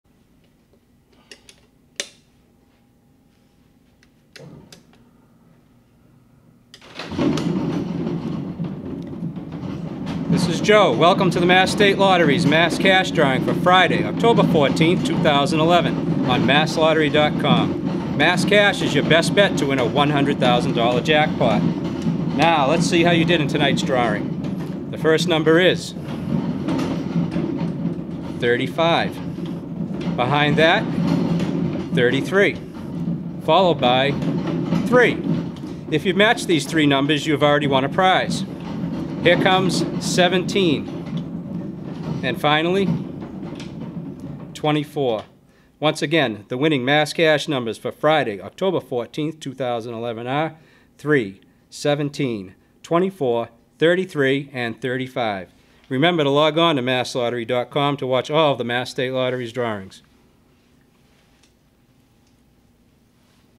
Mass Cash Drawing: Friday, October 14, 2011
The official Mass. State Lottery Mass Cash drawing for Friday, October 14, 2011.